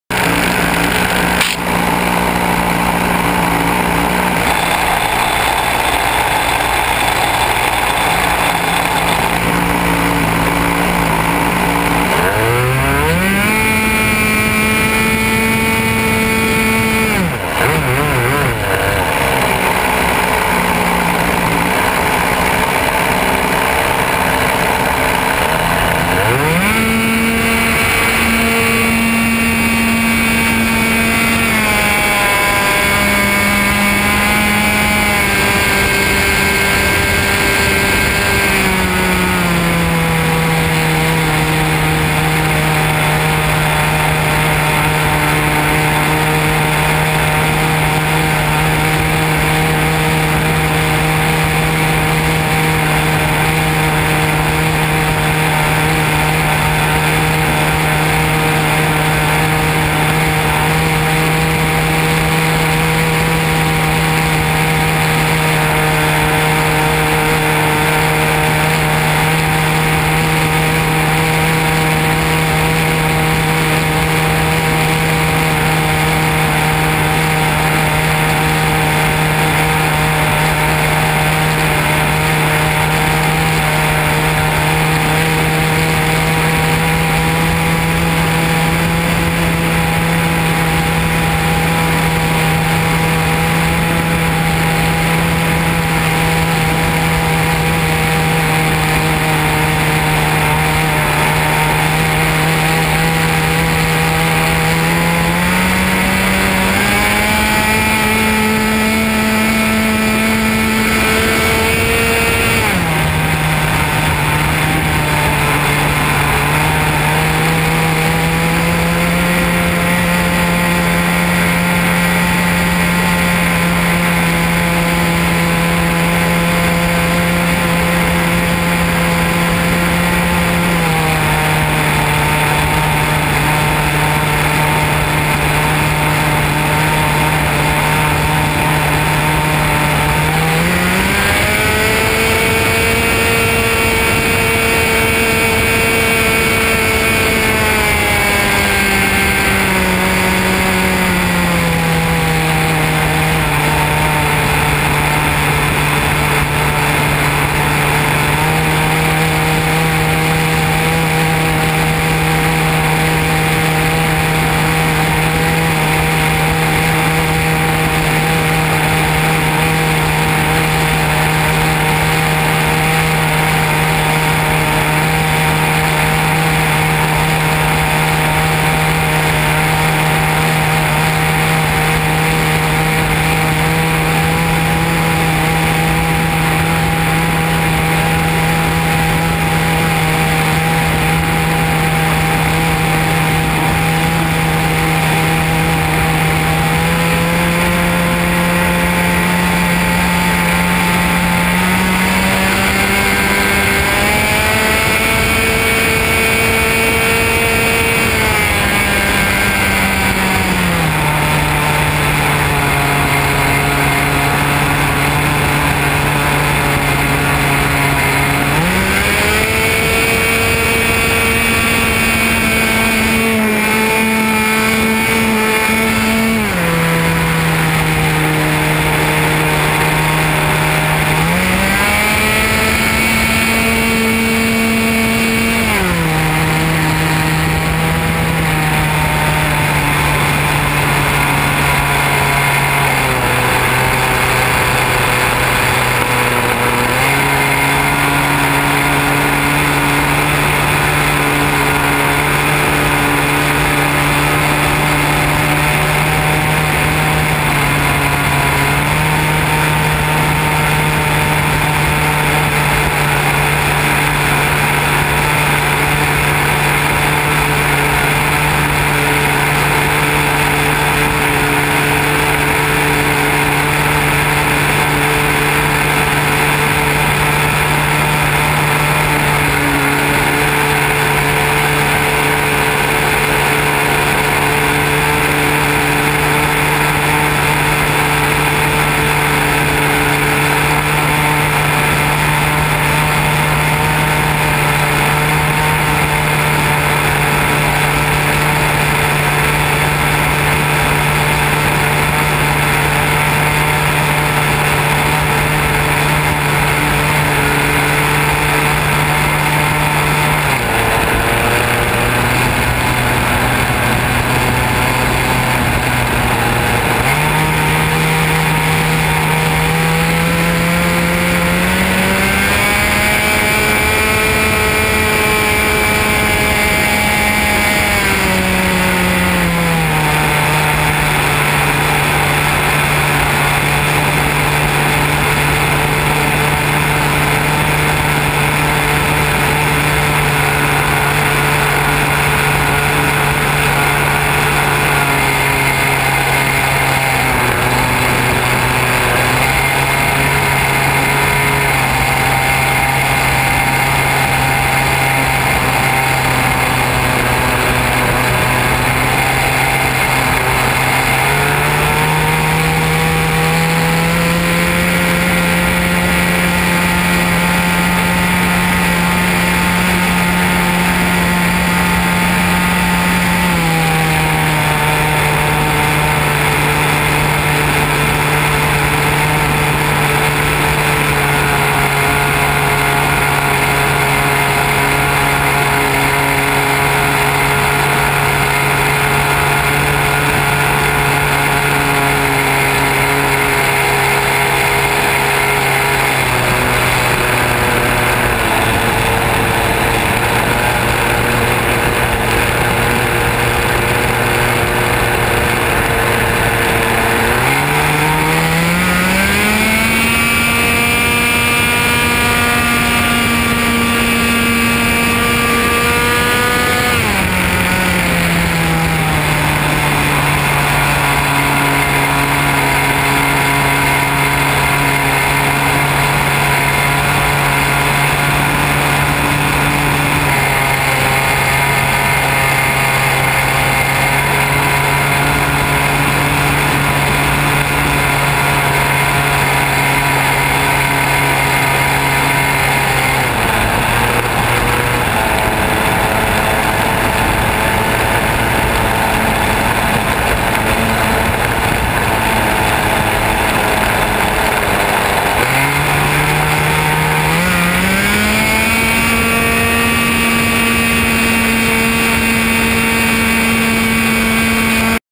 Front View Martha Gopro
Forward view of a go pro camera on a Ms Martha.